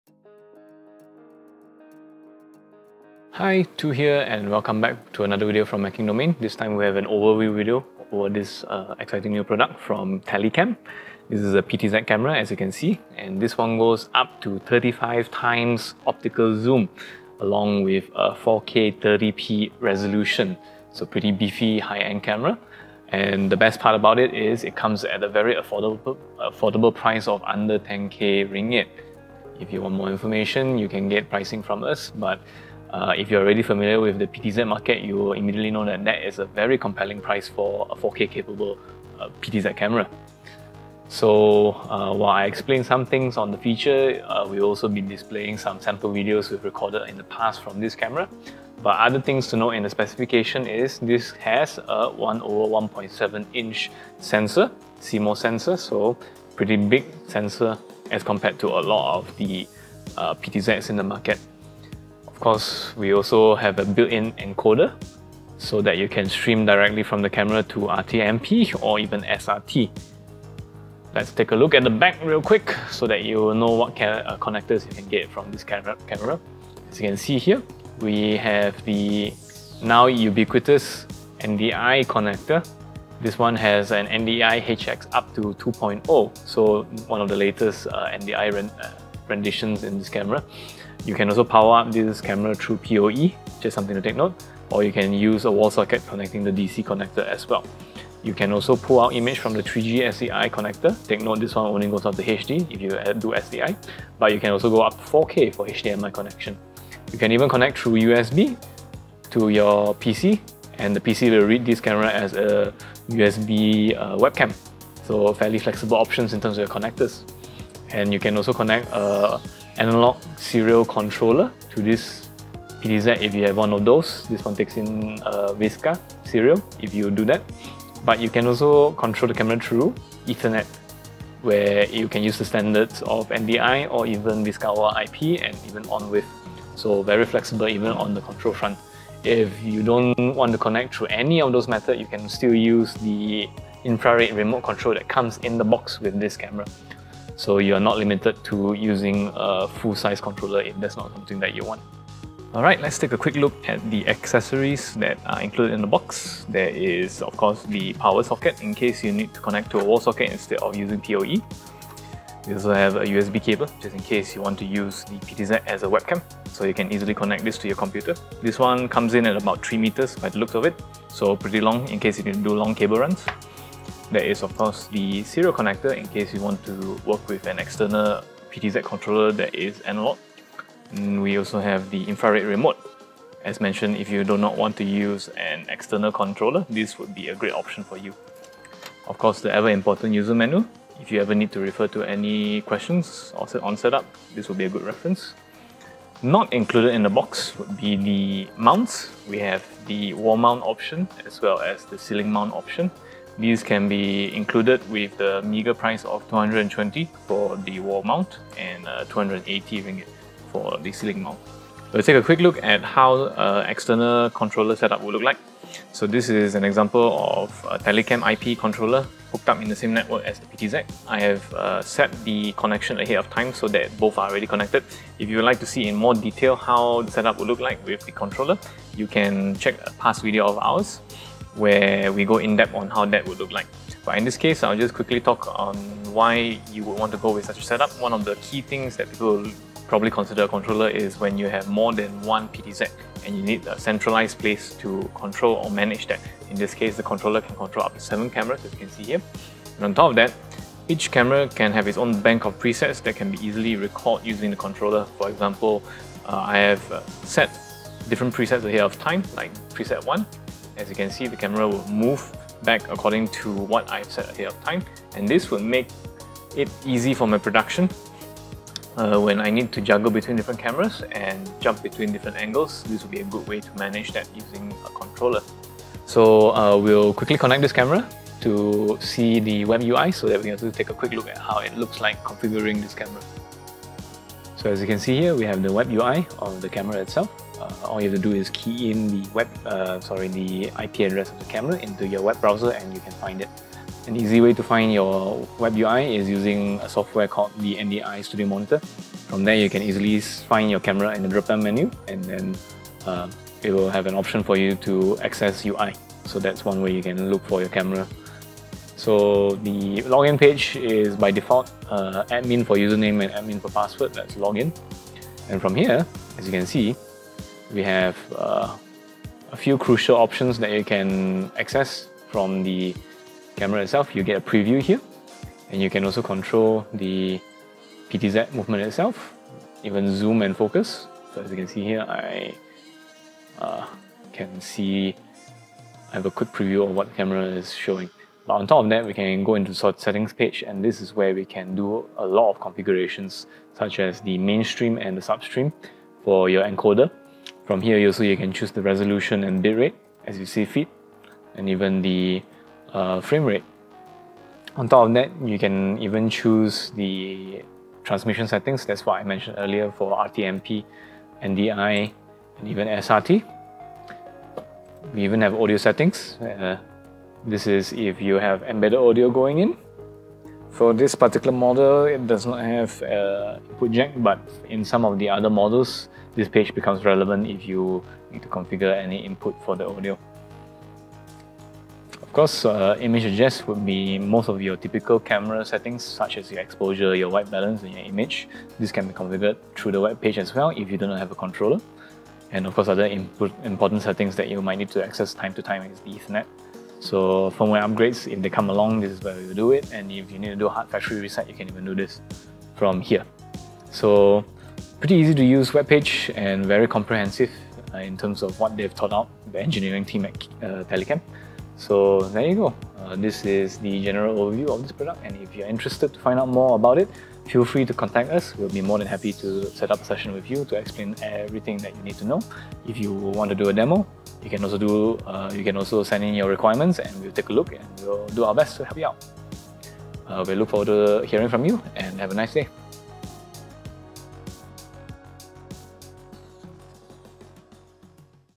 Telycam 35x 4K PTZ Camera Overview | Macking Domain Malaysia
TLC-700-IP-35-4K (NDI) Overview and Demo

In this video we'll take a look at an interesting PTZ camera with great value for quality. With 35x optical zoom and up 4K30p resolution, this camera is more than able to do any production's heavy lifting.

Content timestamps:
0:00 Introduction
0:11 Main features
0:40 Sample footage + features
1:10 Physical connectors (I/O)
2:32 Included accessories + available add-ons
3:32 Connecting with Telycam PTZ Controller
4:36 WebUI configuration
6:51 Wrap-up